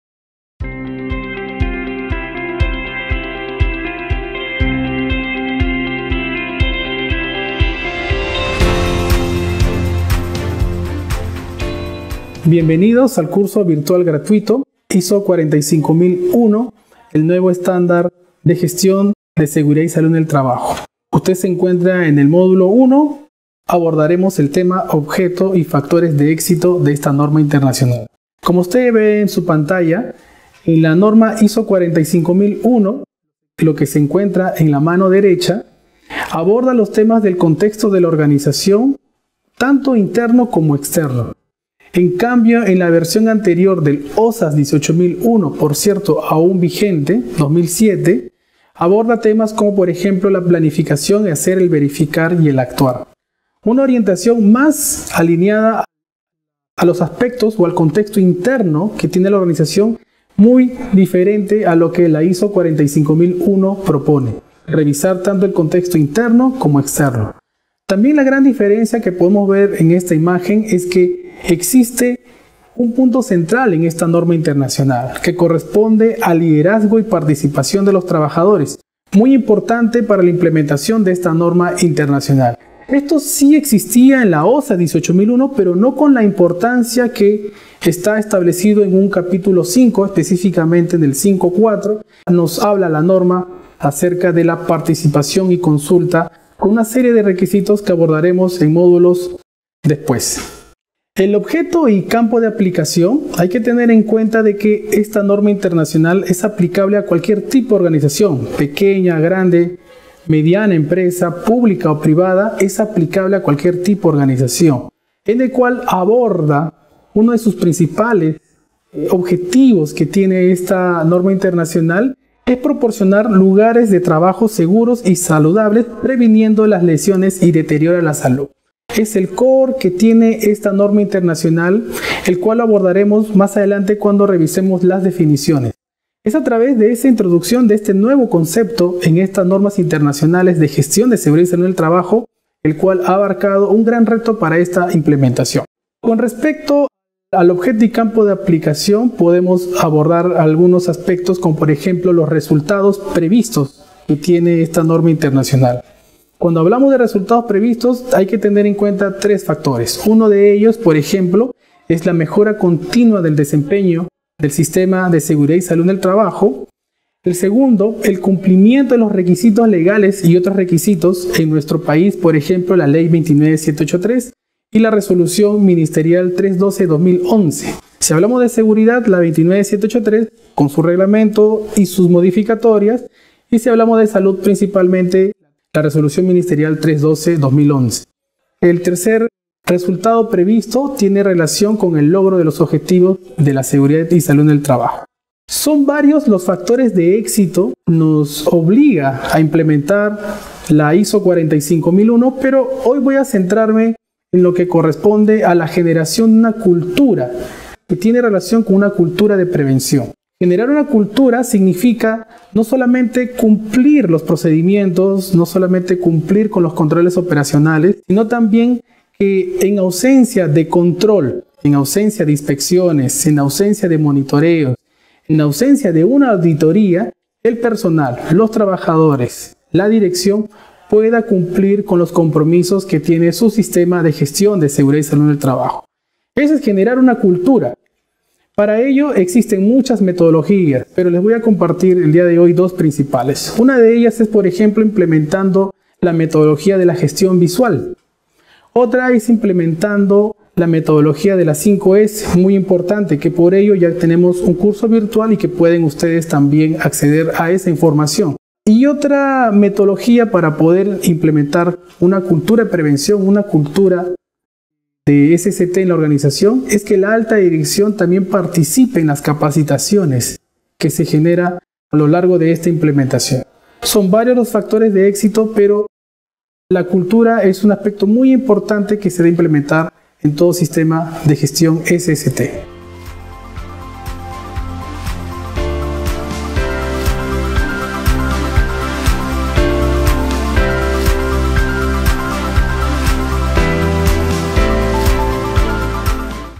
CURSO VIRTUAL GRATUITO: "ISO 45001" - MÓDULO 1.2 - OBJETO Y FACTORES DE ÉXITO PARA ISO 45001
Puedes acceder al Curso Virtual siguiendo el video que ilustra la manera de como inscribirse en la plataforma virtual de Kaizen Virtual.